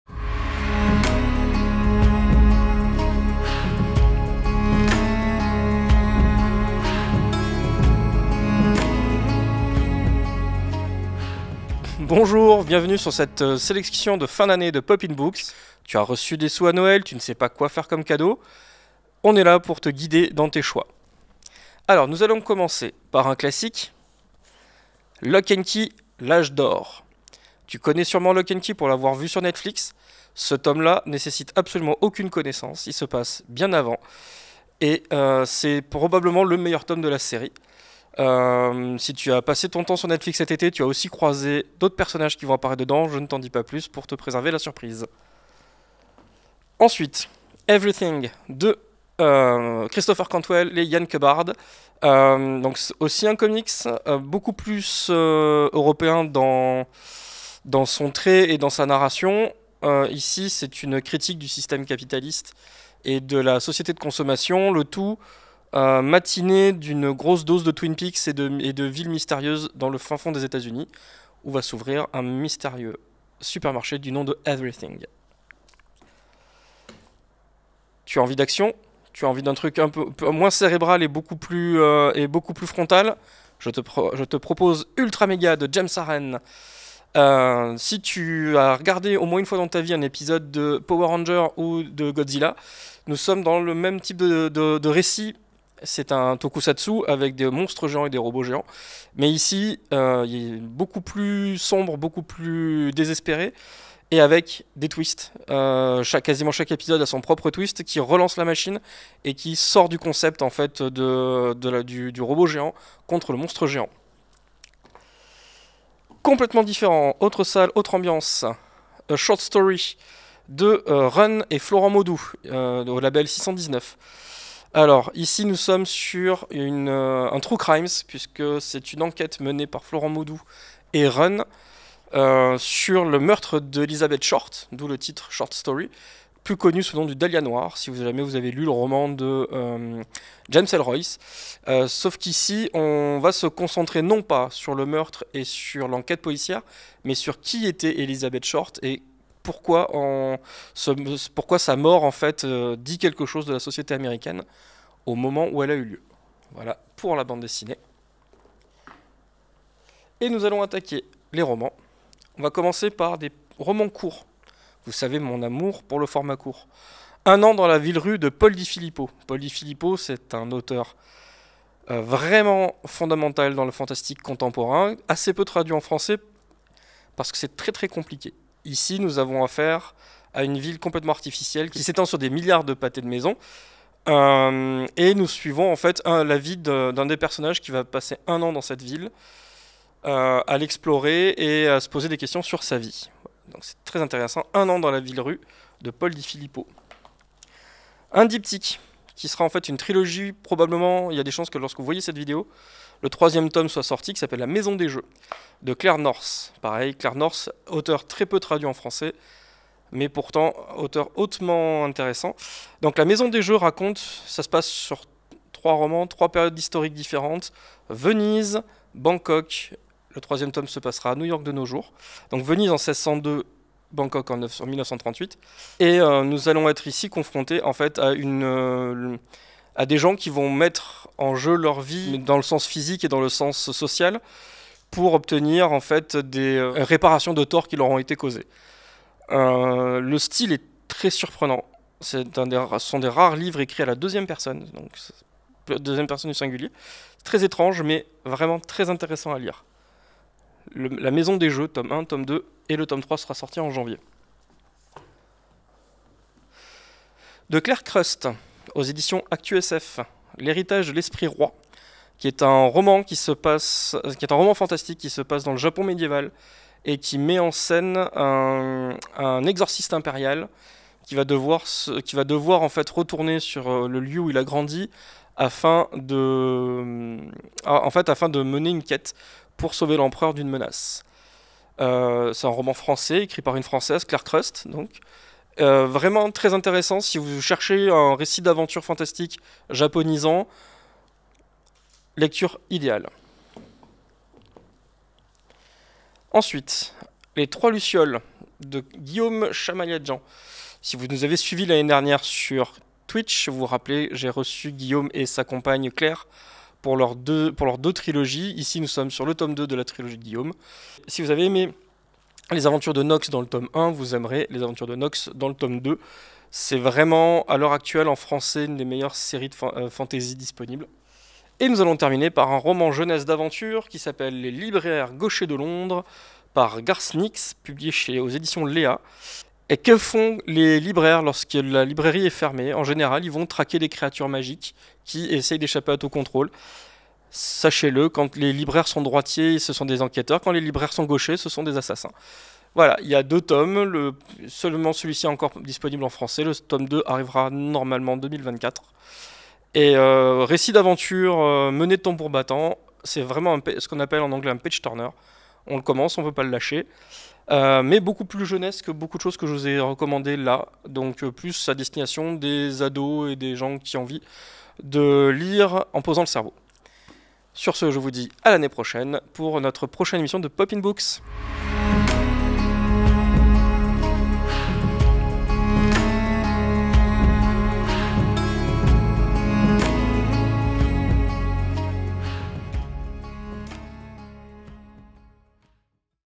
Pop In Books - Sélection lecture 2022
- Pop In Books -
La sélection de Simon Cavalié pour cette fin d'année 2022.

📔 "Locke & Key : L'âge d'or", Joe Hill (éditions Hi Comics)
📔 "Everything", Christopher Cantwell et Ian Culbard (404 Editions)
📔 "Ultramega", James Harren (éditions Delcourt)
📔 "A short Story", Run  (éditions Rue De Sèvres)
📔 "Un an dans la ville-rue", Pierre-Paul Durastanti (Le Belial Eds éditions)
📔"La maison des jeux", Tome 1 : "Le serpent", tome 2 : "Le voleur", Claire North (Le Belial Eds éditions)
📔 "L’héritage de l’esprit-roi", Claire Krust (éditions Actusf)
📔 "Trois lucioles", Guillaume Chamanadjian et Claire Duvivier (Coauteur)  (Aux Forges Vulcain éditions)
📔 "Les libraires gauchers de Londres", Garth Nix (Leha Eds éditions)

Présentation : Simon Cavalié
Montage : Lucie Martin
Production : NEKOPOLIS